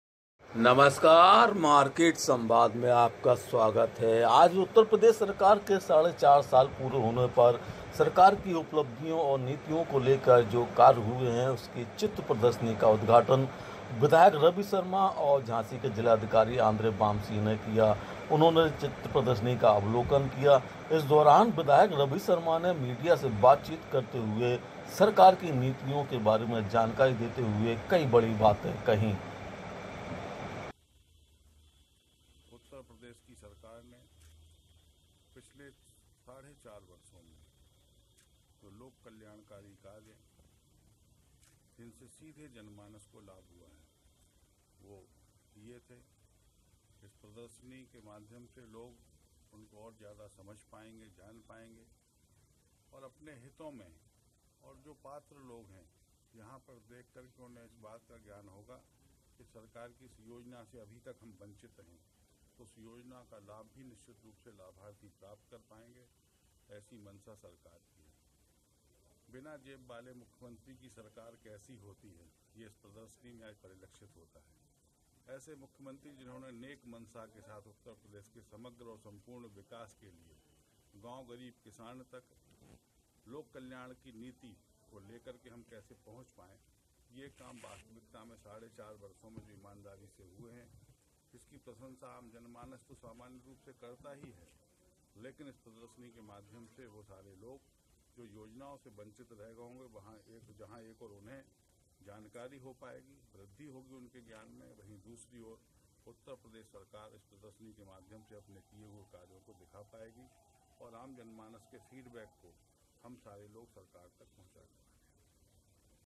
झाँसी: विधायक रवि शर्मा ने क्यों कहा-यह "बिना जेब वाले मुख्यमंत्री" की सरकार है,रिपोर्ट/मनोज दुबे
उत्तर प्रदेश के झांसी में आज प्रदेश सरकार के साडे 4 साल के दौरान किए गए विकास कार्यों की चित्र प्रदर्शनी का उद्घाटन सदर विधायक रवि  शर्मा और जिला अधिकारी आंध्र वापसी ने किया इस दौरान रवि शर्मा ने सरकार की नीतियों के बारे में जानकारी थी